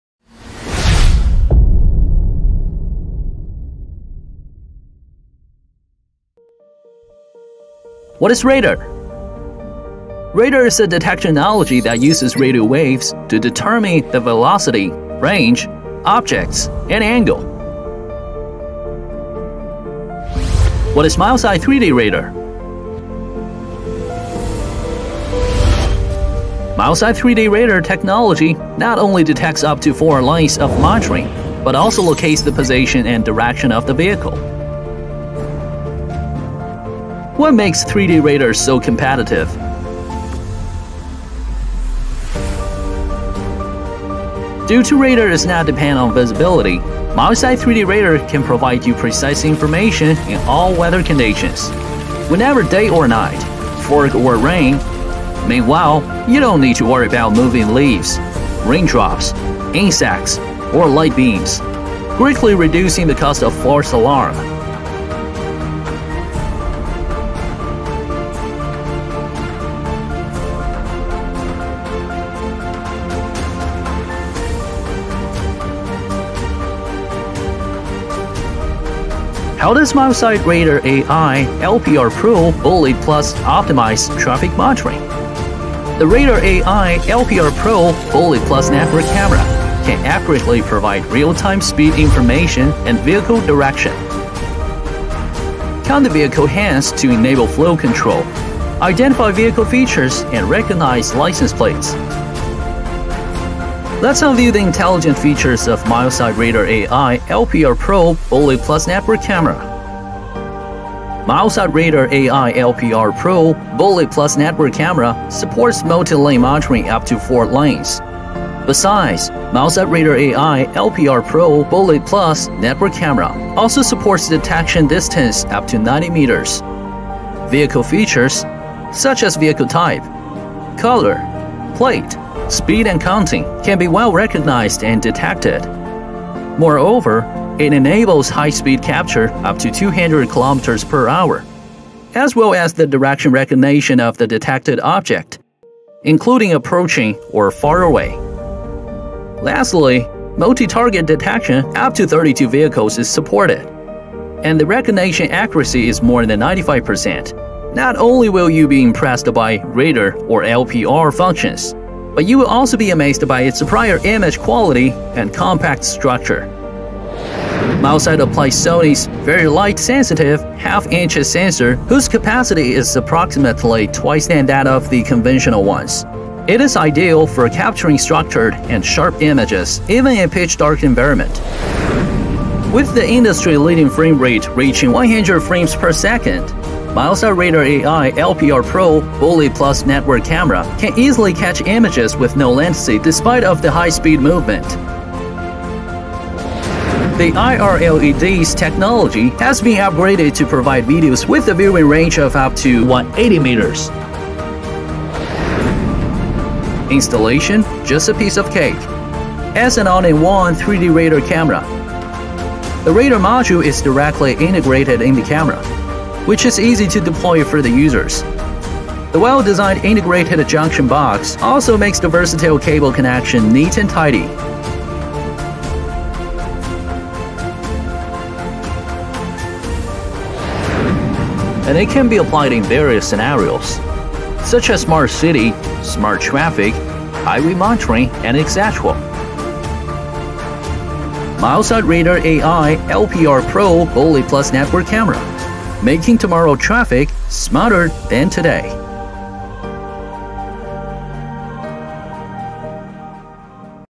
Meet the Milesight Radar AI LPR Camera
Great things come together on the new Radar AI LPR Pro Bullet Plus Network Camera. We combined 3D radar technology, AI-powered LPR algorithm, superior image quality and easy deployment for an intelligent traffic monitoring experience.
Learn more about Milesight Radar AI LPR Pro Bullet Plus Network Camera